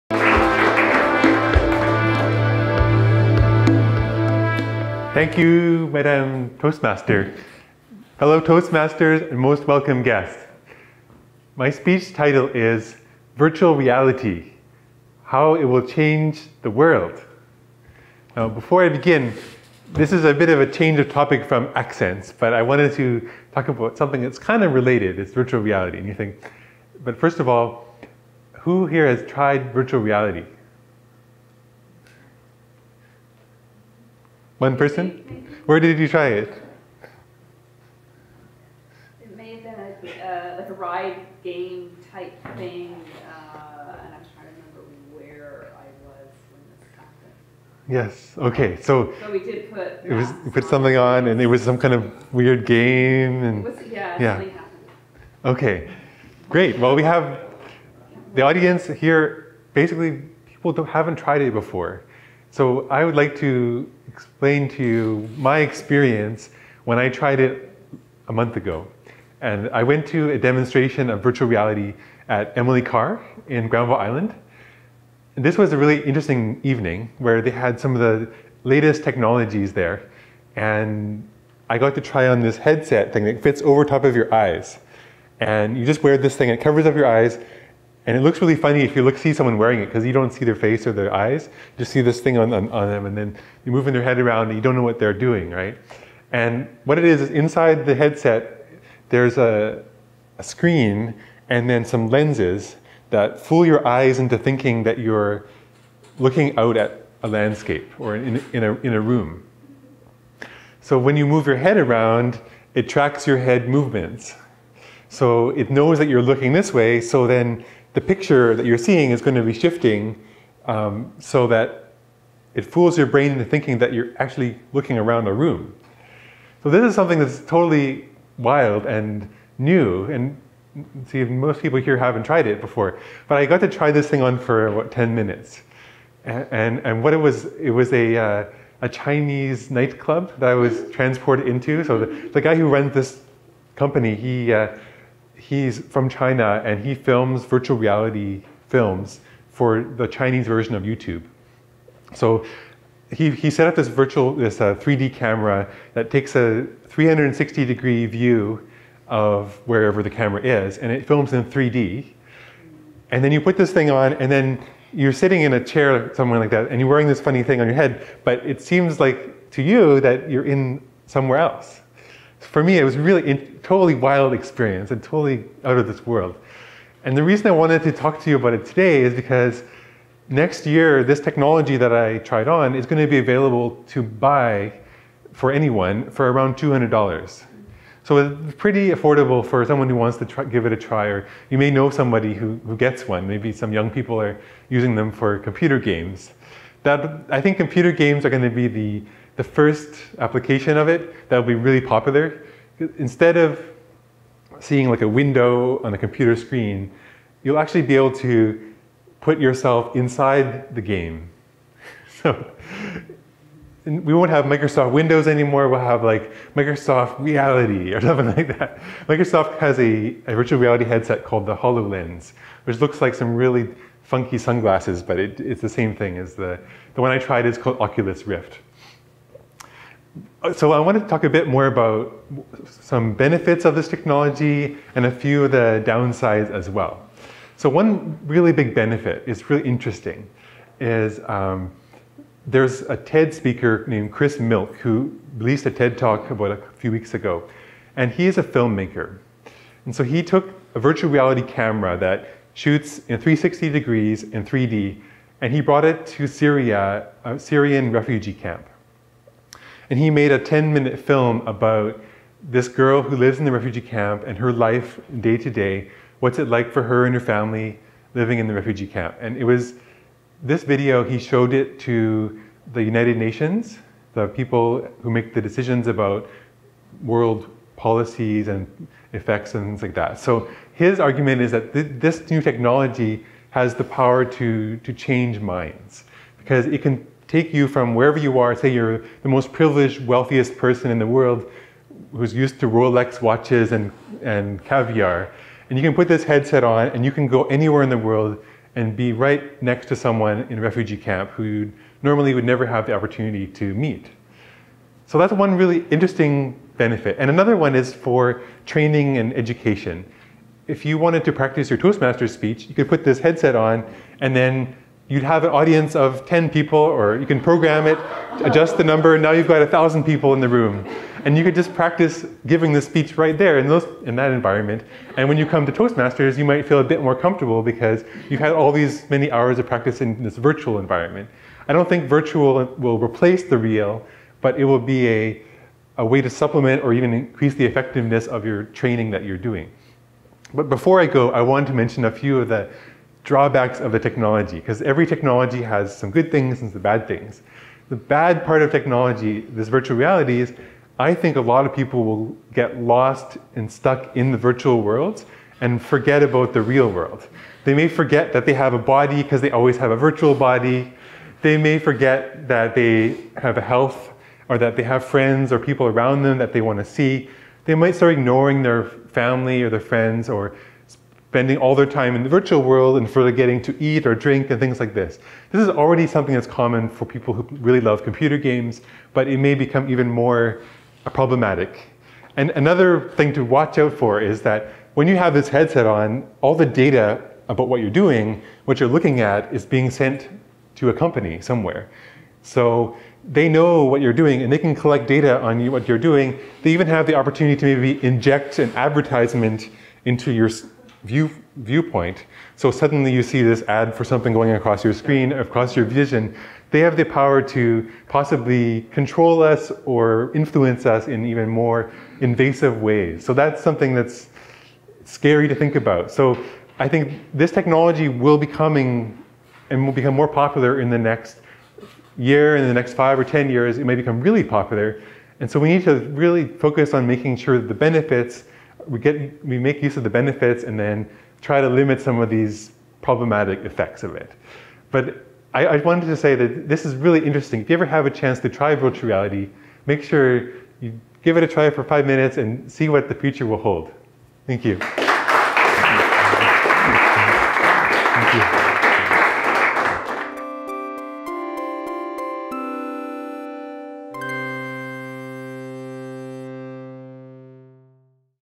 Virtual Reality - How it will change the world - Toastmasters speech by Geoff Peters
gives a speech on Virtual Reality (VR) - How it will change the world.

Recorded on August 24th 2015 in Vancouver BC Canada.

Speech notes: Geoff’s thoughts on Virtual reality

I got to try on the latest Virtual Reality Gear (Oculus Rift Developers Kit 2) a few weeks ago at a demonstration session at Emily Carr in Vancouver.

I wore the headset for 10 minutes and the biggest thing I learned is that I actually felt like I was somewhere else.

Wearing headphones and a virtual reality headset, I was transported into a dark Chinese night club with a DJ, dancers, and people sitting around. When I took the headset off, it was a sudden shock to now be back in a sunny room in Vancouver.

The technology has improved greatly since the first demos of the early 90’s. Next year, Oculus will be releasing this technology for consumers, and it will be affordable.

It is currently possible to use VR using your smart phone and a headset such as Google Cardboard or Oculus Gear VR.

The biggest thing I realized is that VR can transport you into another place. By tracking your head movements and changing the image as you move your head, it fools your brain into thinking you are in a virtual environment.

Filmmakers are now filming with 360 degree 3D cameras, such as TED speaker Chris Milk’s VR documentaries about refugee camps in Syria.

Instead of looking at our computer screens through the “window” of the monitor, VR will put us right inside the world of the computer game, filmmaker, or simulated environment.

One example where VR could be very useful is in education, for students and teachers. Imagine that you are preparing for a Toastmasters speech or a business presentation: you could wear a VR headset and get practice giving your speech in front of a virtual audience of 10 to 1000 people.

I think that when learning a new skill, a combination of virtual world practice and real world practice will be more effective than exclusively virtual or exclusively real world practice.

Like any technology, VR will come with problems of its own. There is a danger of getting lost in a virtual world and neglecting the real world, and causing people to become less mindful about their actual bodies and real world relationships. Also, corporations will continue collecting data about us as we use Virtual Reality headsets, and have the potential to use even more invasive strategies to market to us or control us.

A great science fiction book that I recently read that explores the topics of virtual reality and augmented reality is called “Rainbows End” by Vernor Vinge. It delves into the impact of virtual reality on our education system, daily life, and world conflicts / politics, and is a great read.

Virtual reality is definitely a technology to watch, and will have a huge impact on how we live our lives over the next 10 years. Exciting times ahead!